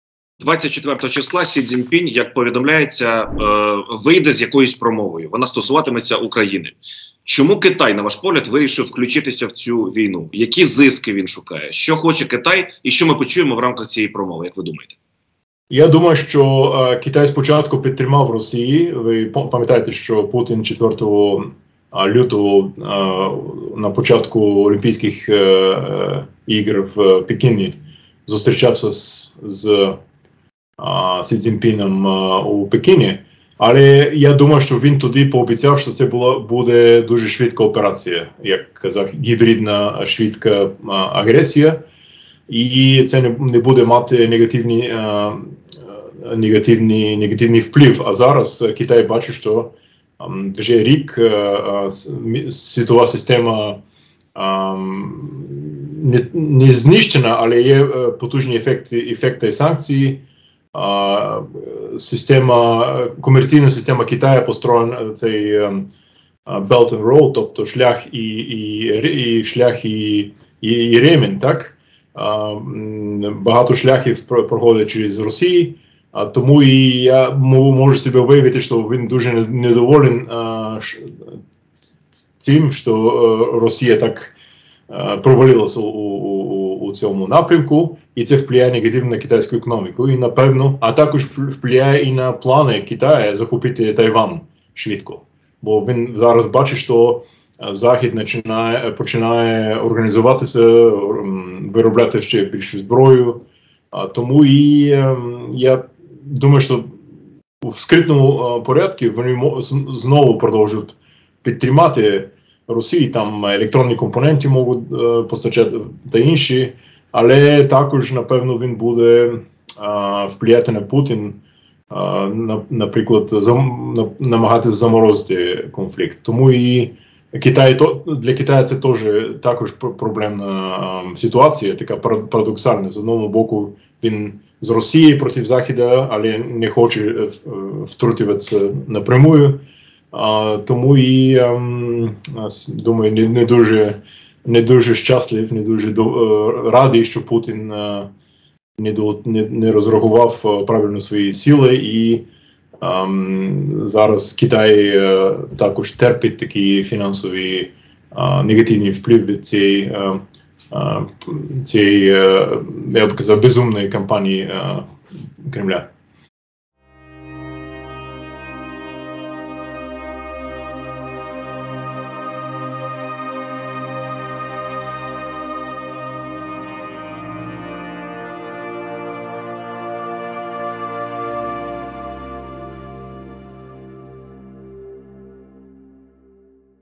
🔥ЖЕСТЬ! КИТАЙ дуже ЗЛИЙ на ПУТІНА, на пусті ОБІЦЯНКИ диктатора буде СЕРЙОЗНА ВІДПОВІДЬ — Войджер
Американський військовий аналітик і колишній радник командувача армії США в Європі Марк Войджер в інтерв’ю Юрію Бібіку розповів про роль Китаю у війні Росії з Україною та яку позицію обере президент КНР Сі Цзіньпінь.